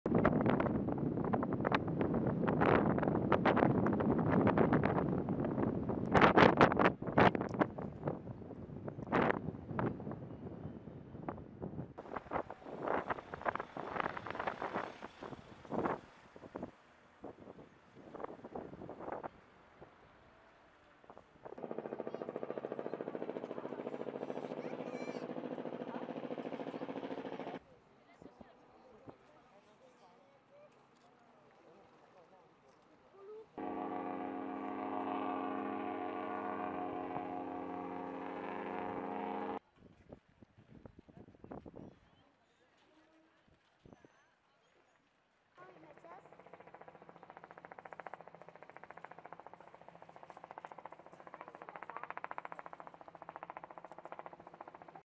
Le Celebrity Edge quitte Saint-Nazaire pour les Etats-Unis
Long de 306 m, le Celebrity Edge, palace flottant ultra-luxueux et innovant, capable d'accueillir 2.900 passagers et 1.320 membres d'équipage, quitte le port de Saint-Nazaire pour rallier les Etats-Unis. Ce paquebot de croisière a été fabriqué dans les cales des Chantiers de l'Atlantique pour l'armateur américain Royal Caribbean Cruise, déjà propriétaire du plus grand paquebot de croisière du monde : le Symphony of the seas. IMAGES